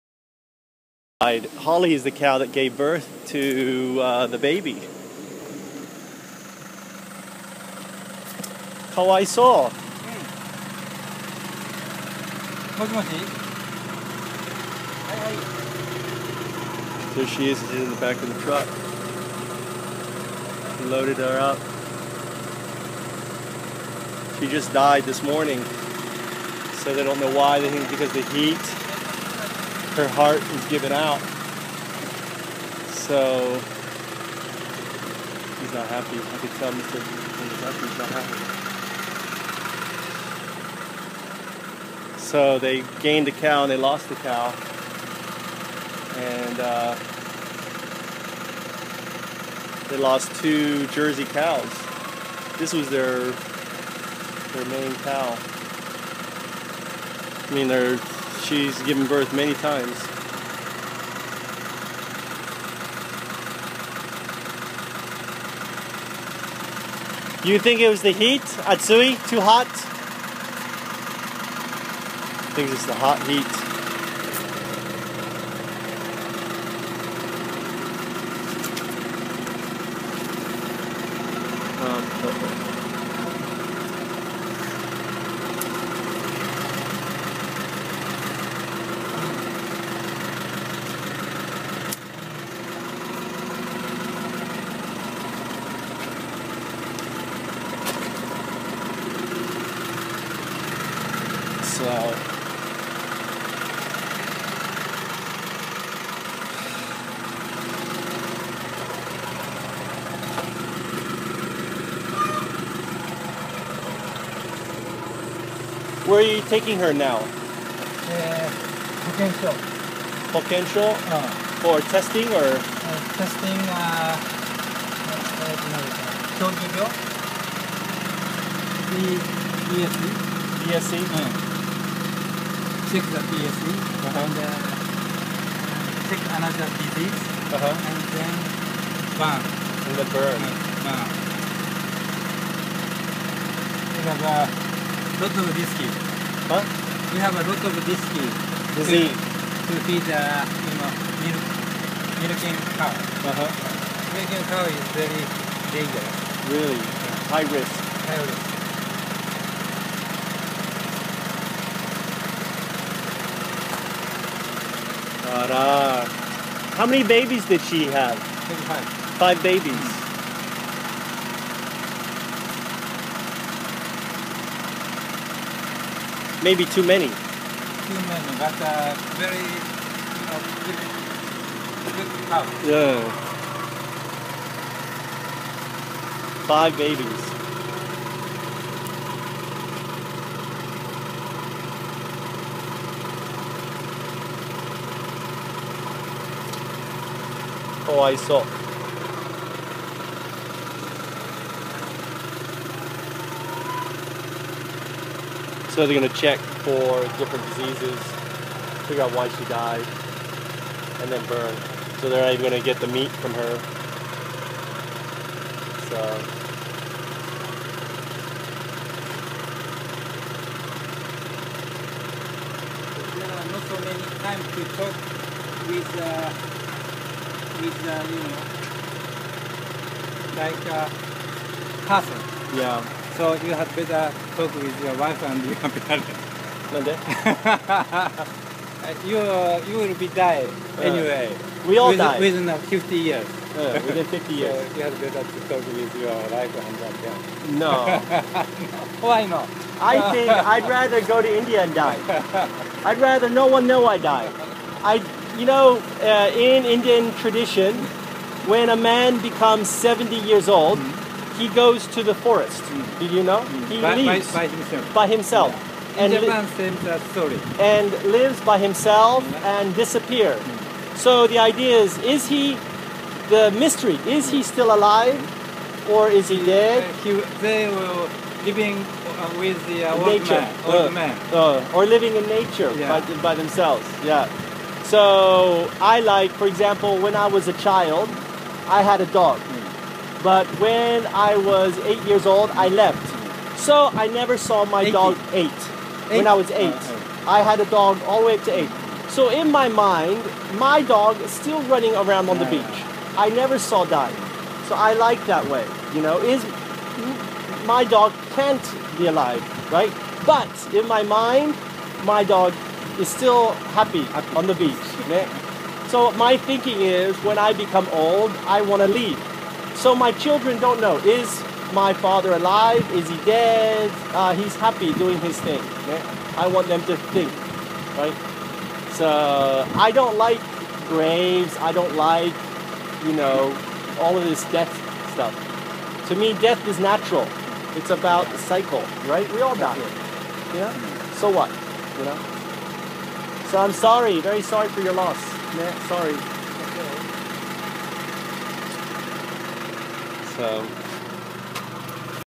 Holly died today... Okera Farm's prize Jersey Cow died after giving birth
October 31, Holy, Okera Farm prize Jersey cow died suddenly after giving birth. Mr. Okera and I talk about death and I share my ideas on death.